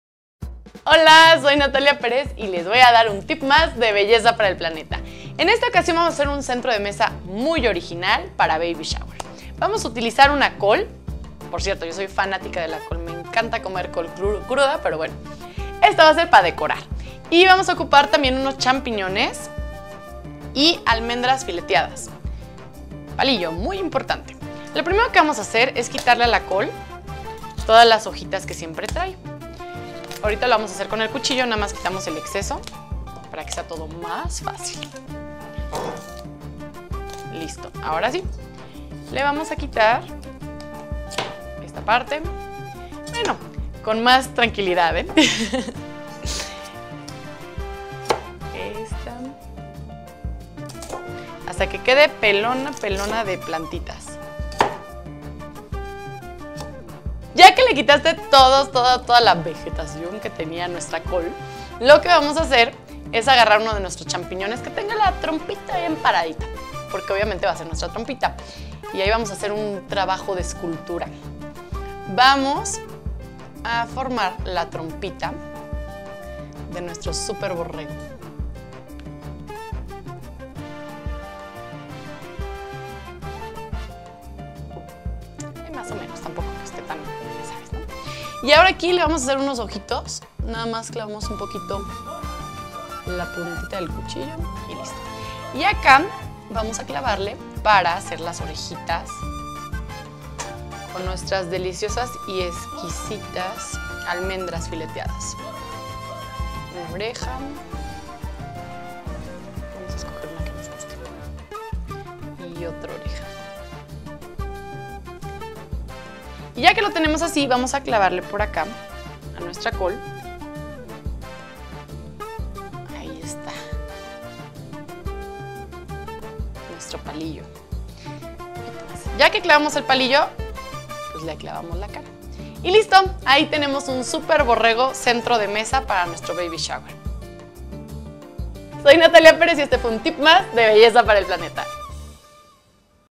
Borrego de coliflor
Natalia Pérez nos enseña cómo hacer un centro de mesa en forma de borrego con una coliflor.

Visita para conocer más sobre los pañales Bio Baby.